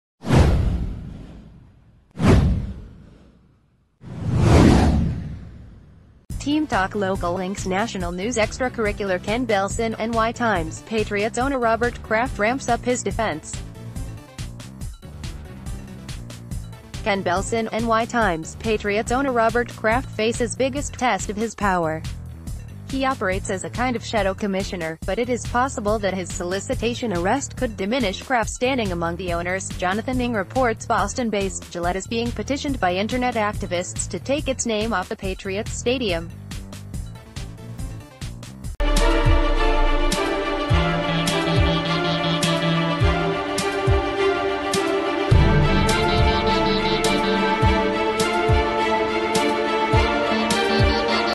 New England Patriots links 3/06/07 - Sony Michel enjoys downtime after full year of full go
TEAM TALK LOCAL LINKS NATIONAL NEWS EXTRACURRICULAR Ken Belson (NY Times) Patriots owner Robert Kraft ramps up his defense. Ken Belson (NY Times) Patriots owner Robert Kraft faces biggest test of his power. “He operates as a kind of shadow commissioner, but it is possible that his solicitation arrest could diminish Kraft’s standing among the owners.” Jonathan Ng reports Boston-based Gillette is being petitioned by internet activists to take its name off the Patriots stadium.